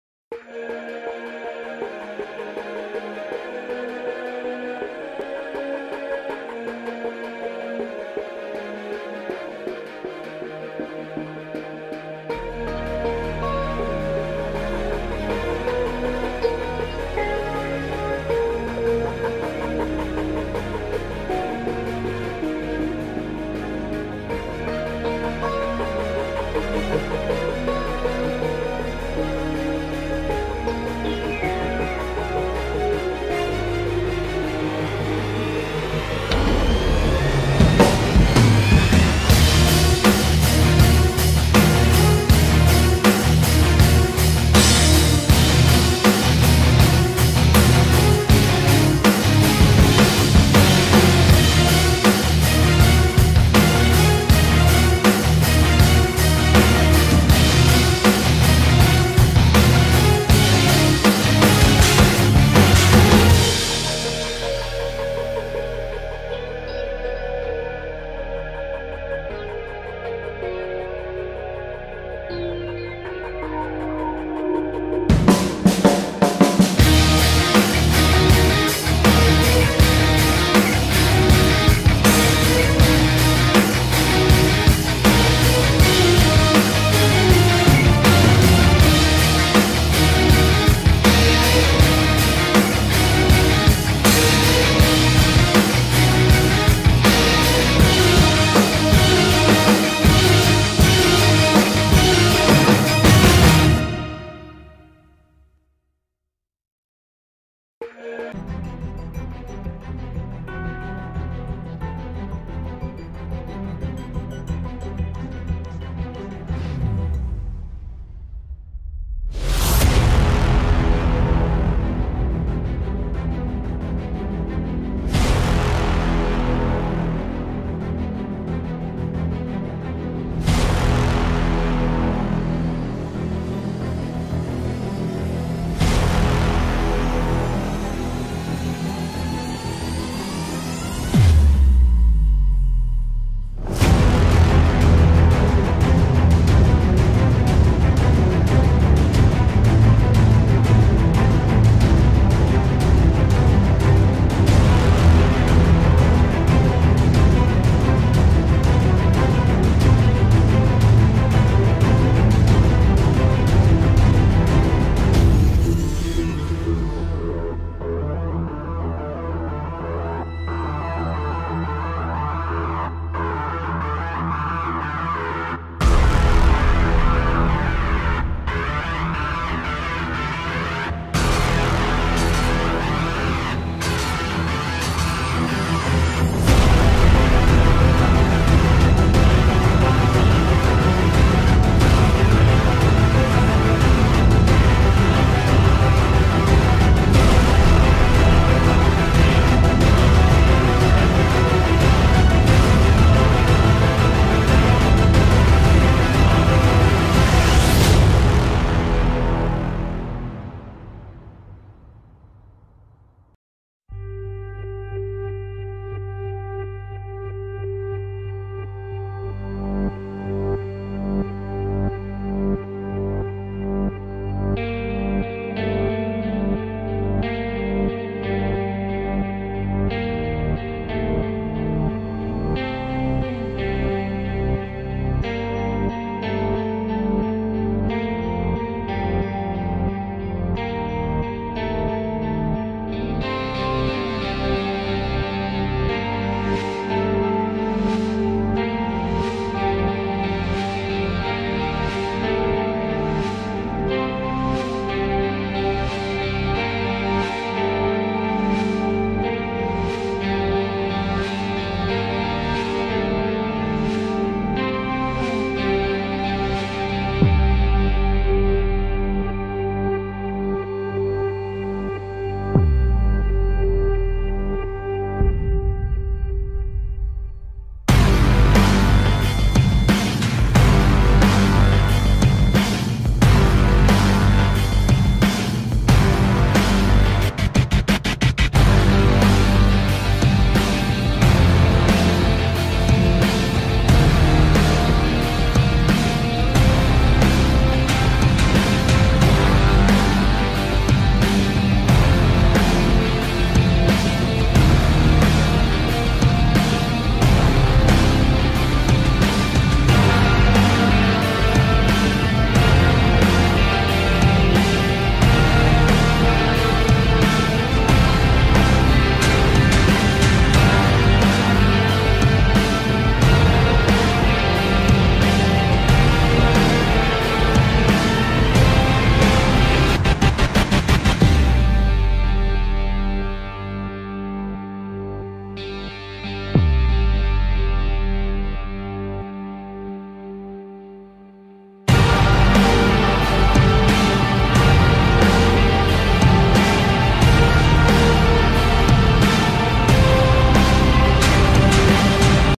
THE FINAL IS HERE ((Recap of the Tournement))
I'm sorry i didn't add my voice into this video, it kept messing up and i figured the video was better with Music instead of well ruining the video with out of sync voice overs, Sorry. Hope you enjoy it anyway and tune in for the final